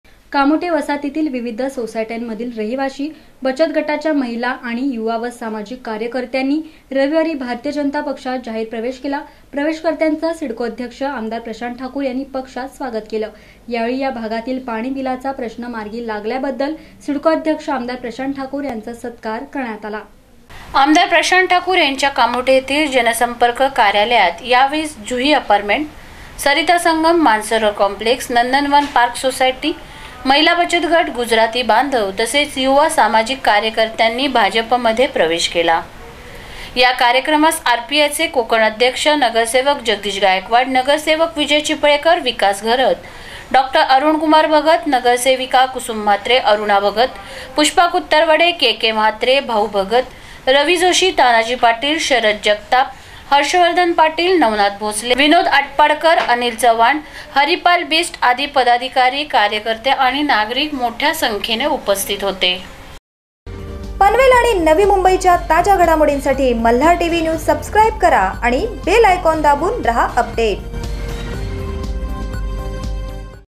कामोठ्यातील विविध सामाजिक कार्यकर्त्यांचा भाजपात प्रवेश !
जिल्हाध्यक्ष आमदार प्रशांत ठाकूर यांच्या उपस्थितीत प्रवेश !